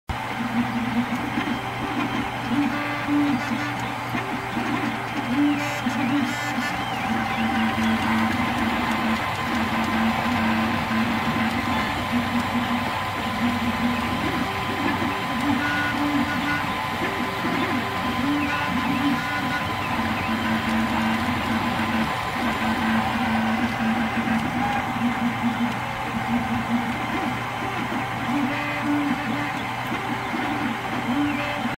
RepRap MendelMax 2.0 3D Printer Printed Parts Production
Printing components for the MakersTookWorks MendelMax 2.0 3D printer.